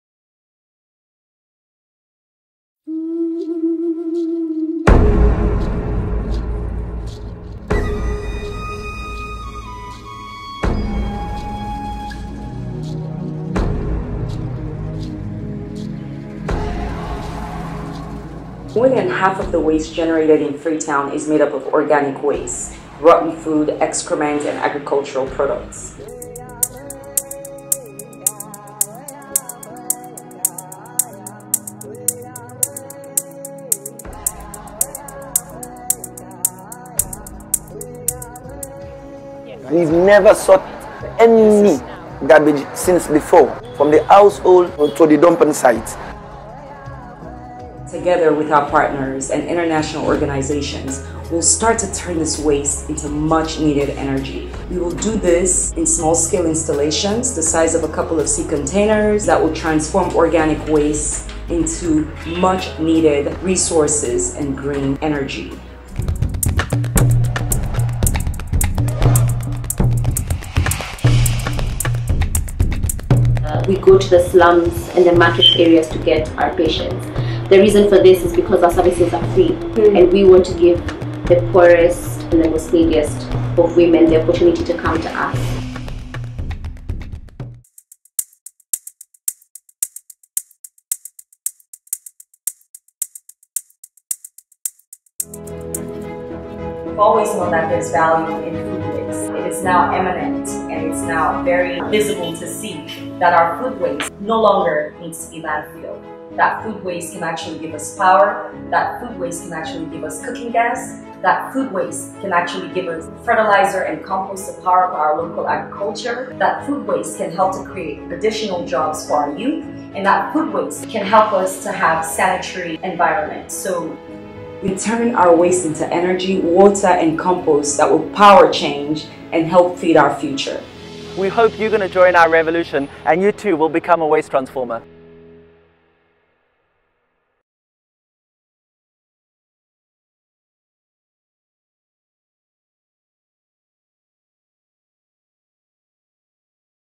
Powering a local hospital on organic waste in Freetown | The Waste Transformers
The capital of Sierra Leone, Freetown is home to over one million people. The waste produced in this city is mainly organic waste, 84% in total. Most of the waste ends up on the street, floods into the waterways, or on landfills, where it is left to rot and emits methane. The Waste Transformers partnered up with the local waste management company to place multiple Waste Transformers (anaerobic digesters) in Freetown to turn the organic waste into clean energy and a natural fertilizer.

This Waste Transformer turns the organic waste of the neighbourhood into energy, the energy is sold to the local hospital with a 10% reduced electricity rate.